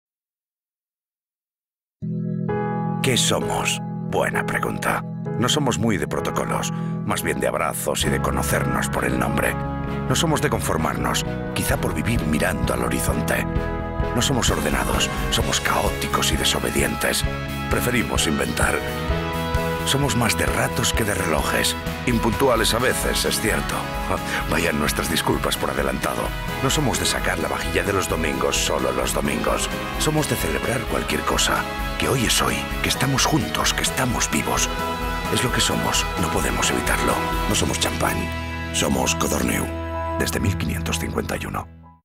Manifiesto | No somos champagne. Somos Codorníu.
¿Qué somos? No somos muy de protocolos; más bien, de abrazos y de conocernos por el nombre. No somos de conformarnos, quizá por vivir mirando al horizonte. No somos ordenados. Somos caóticos y desobedientes. Preferimos inventar. Somos más de ratos que de relojes. Impuntuales, es cierto. Vayan nuestras disculpas por adelantado. No somos de sacar la vajilla de los domingos solo los domingos. Somos de celebrar cualquier cosa: que hoy es hoy, que estamos juntos, que estamos vivos. Es lo que somos; no podemos evitarlo.
No somos champagne. Somos Codorníu.
Desde 1551.